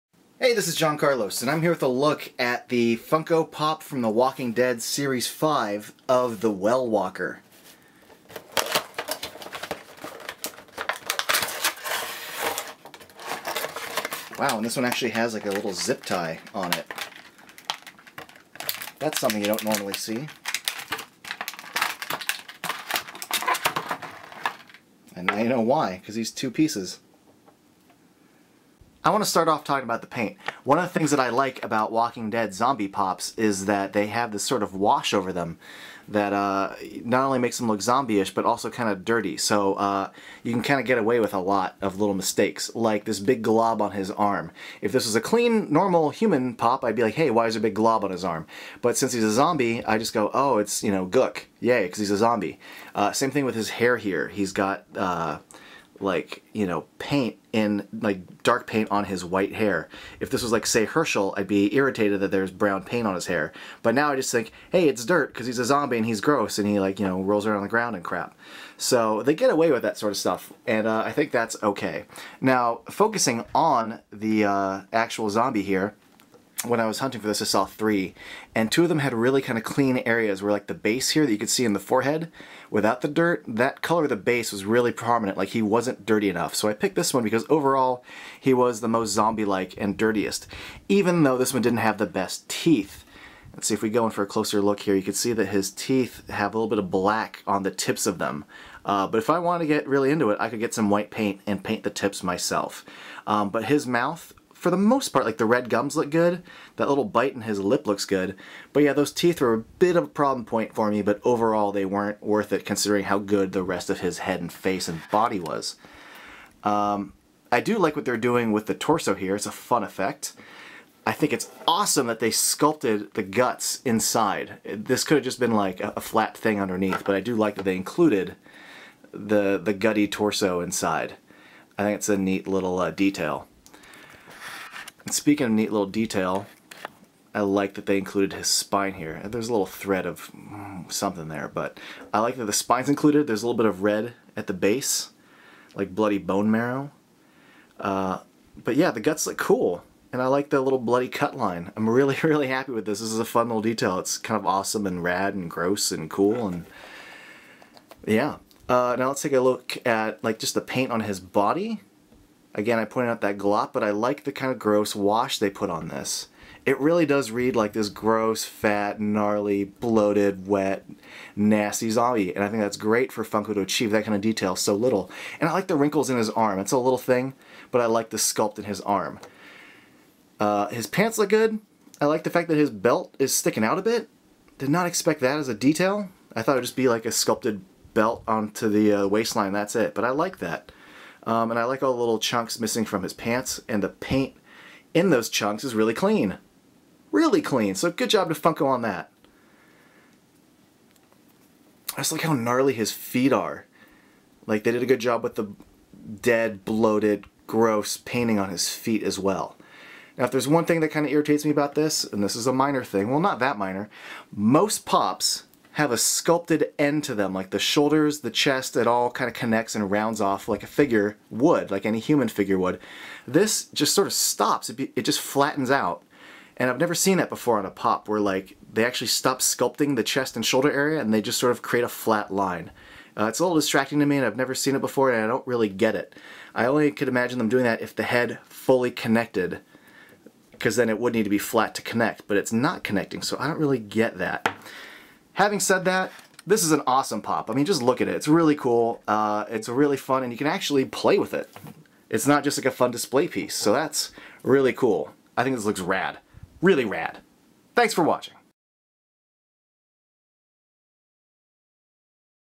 Well Walker The Walking Dead Funko Pop review
Unboxing & review of the Well Walker The Walking Dead series 5 Funko Pop! vinyl figure. by John Carlos McMaster.